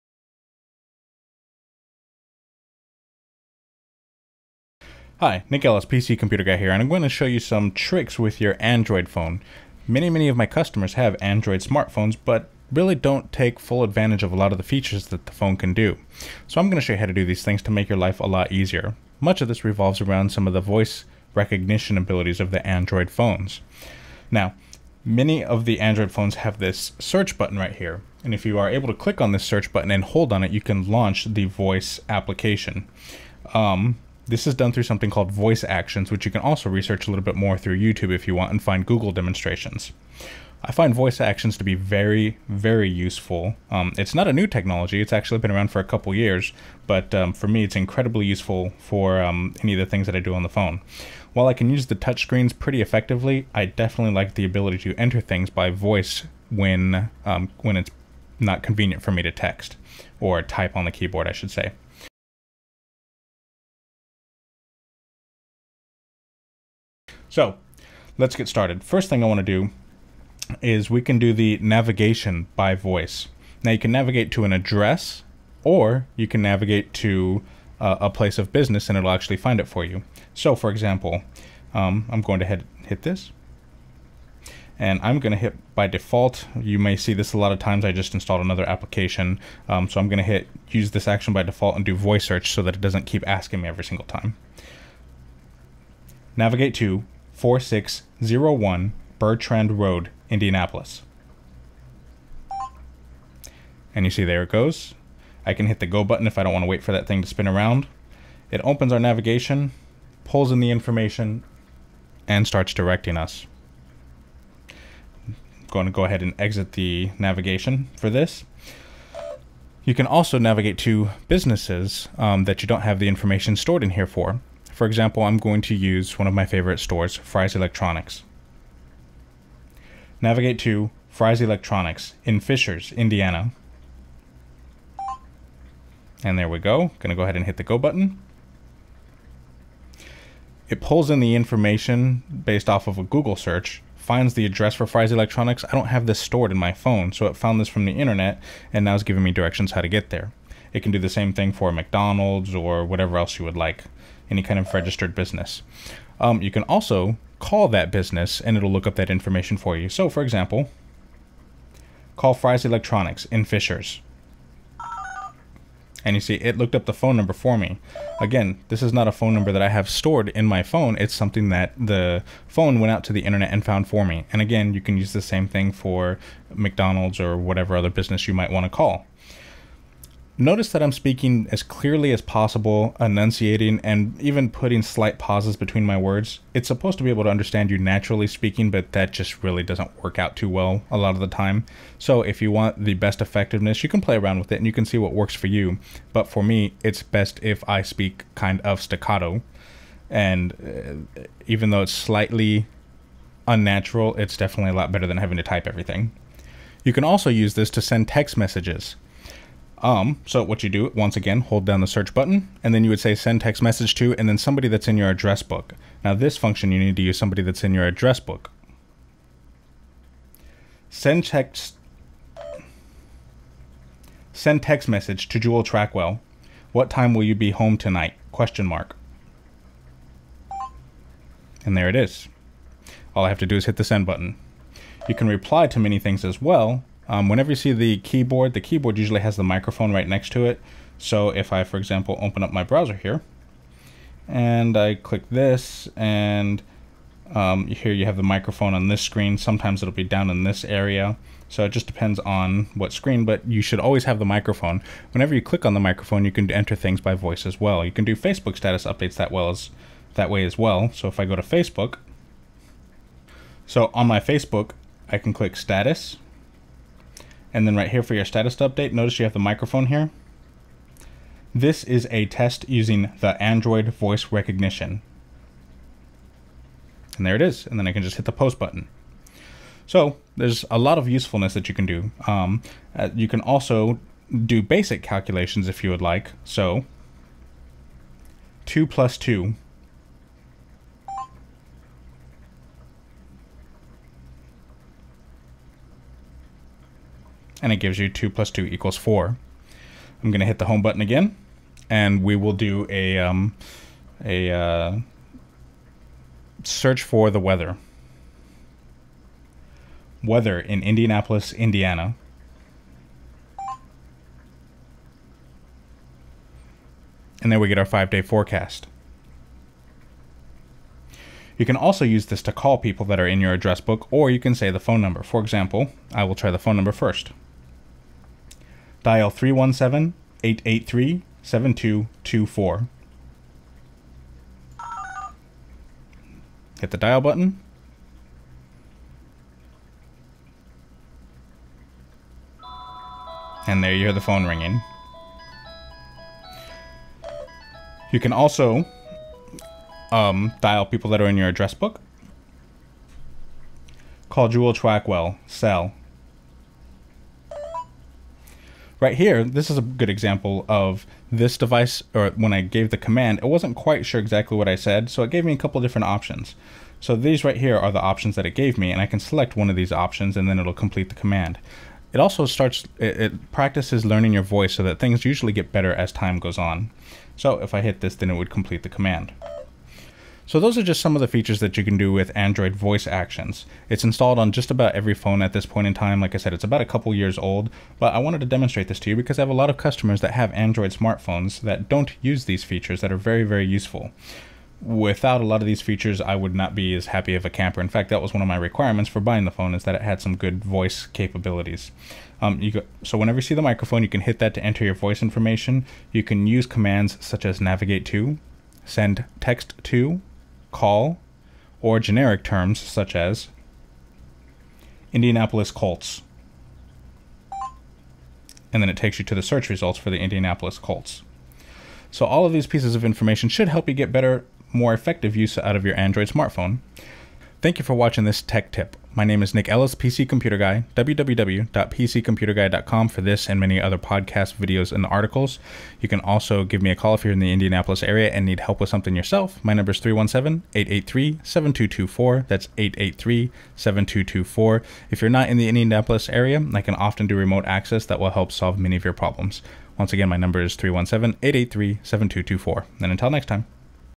Android Voice Actions with PcComputerGuy
Many people have Android Smartphones, but don't make smart use of them! In this video I demonstrate some of the 2 year old voice actions that Android supports to make your life much easier! Not shown in this video is also the "Listen to.." command, which you can then say an Artist or Song and select how you want to listen to that choice.